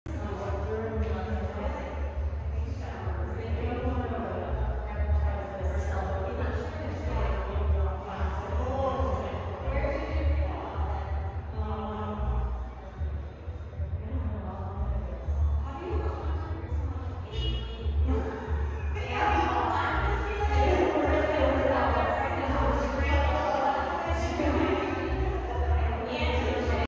Mary Curtis Ratcliff - Bay Fugue
Mary Curtis Ratcliff's kinetic sculpture "Bay Fugue." April 2021.